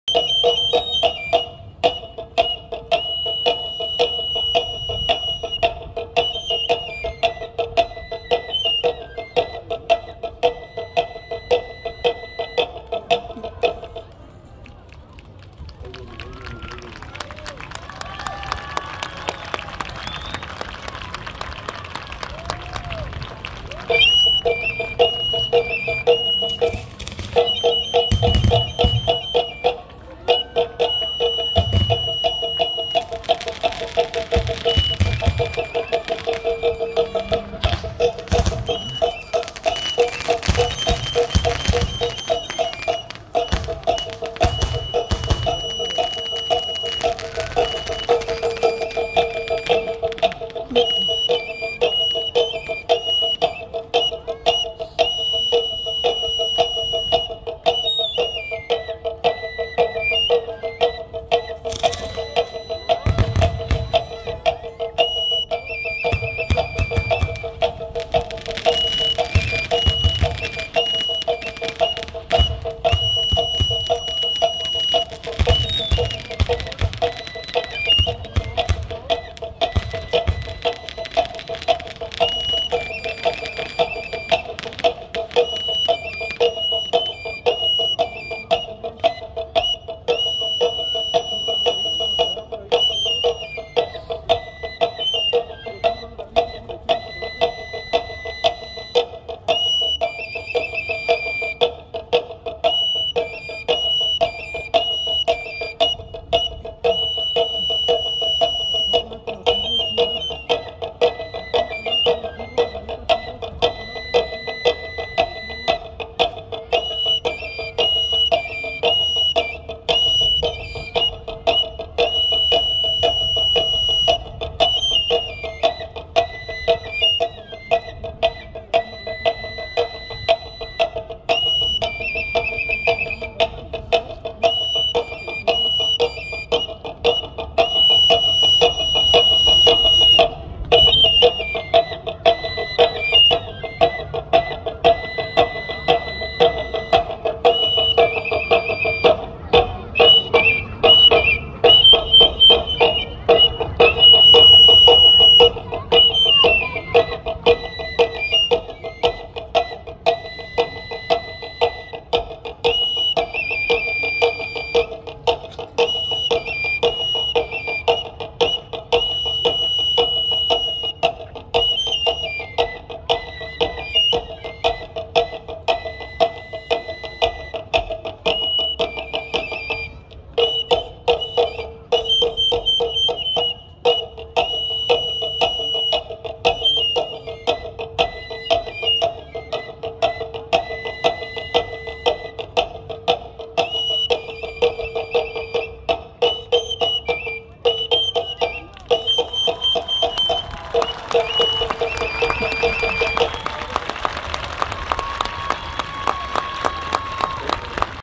Danza de los Voladores - video completo
Danza de los Voladores de México. Grabando en Atlixco, Mexico. Video completo.